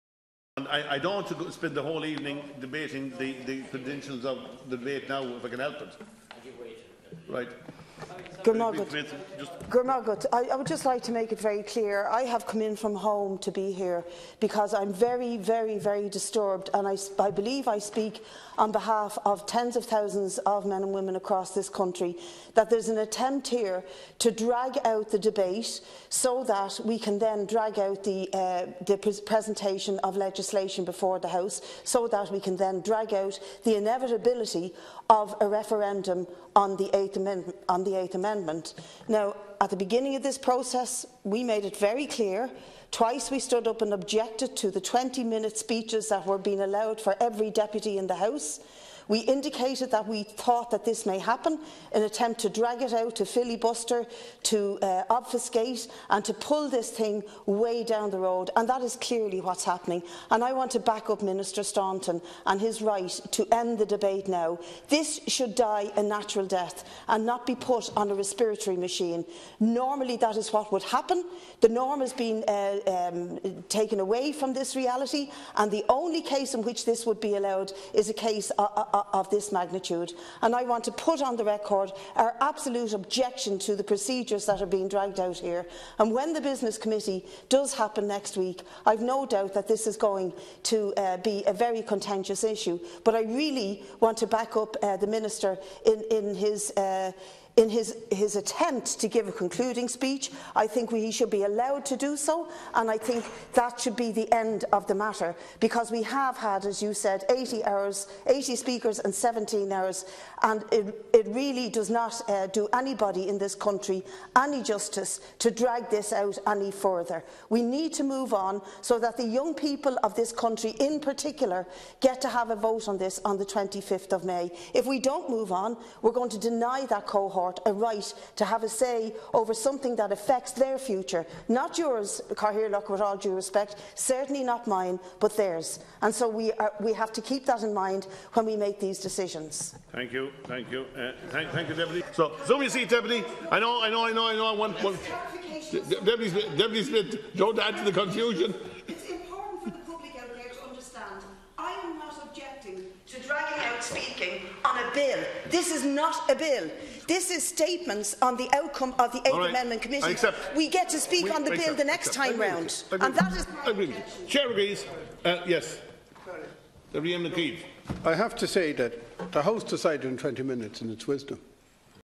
Brid Smith TD; anti-choice filibuster attempt Dail, 25.1.18
It was outrageous. The Anti-Choicers in the Dáil tonight attempted to drag out the debate after more than 80 speakers, and 17 hours speaking time, and when the Minister rightly tried to draw it to a conclusion, they continued 'messing'. Why? By dragging things out it could end up delaying the referendum and thereby disenfranchising young people – the very ones who will be affected by its outcome.